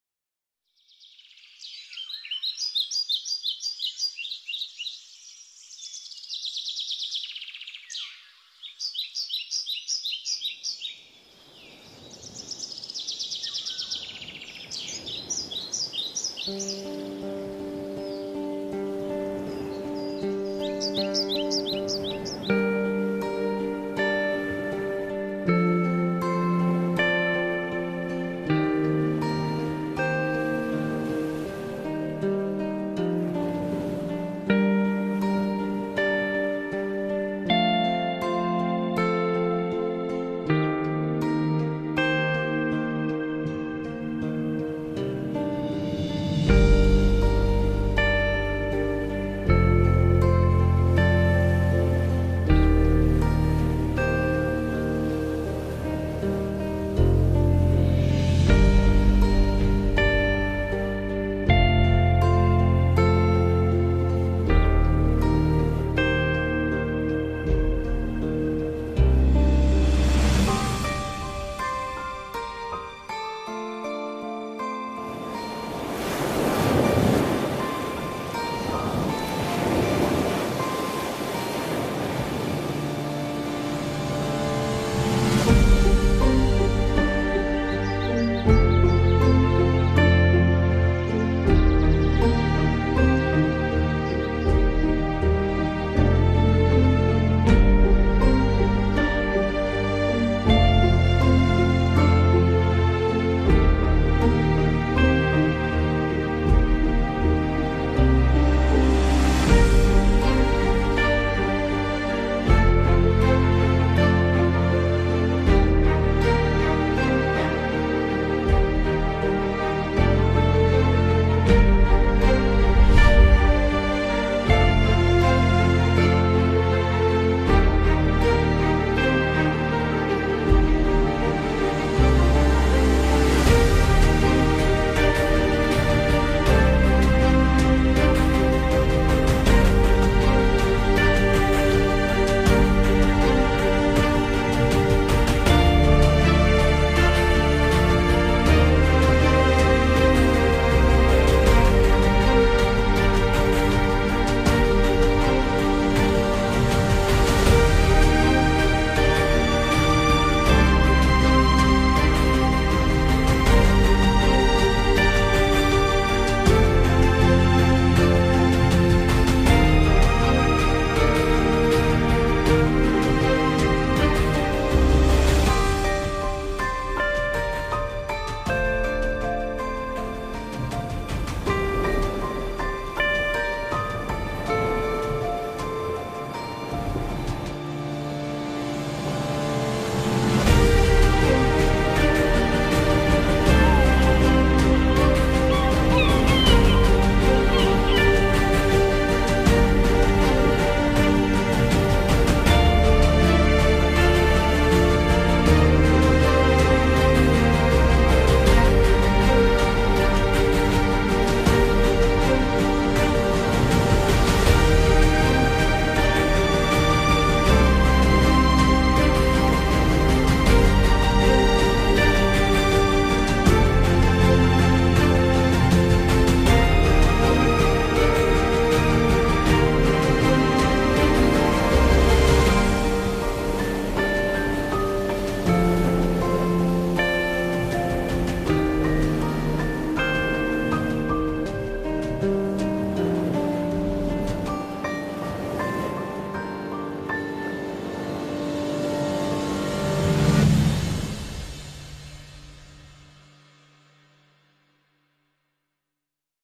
JEJU ISLAND 4K VIDEO DRONE 제주도
After hours and hours of flight and editing, I have finally achieved something good to show. This is Jeju Island in South Korea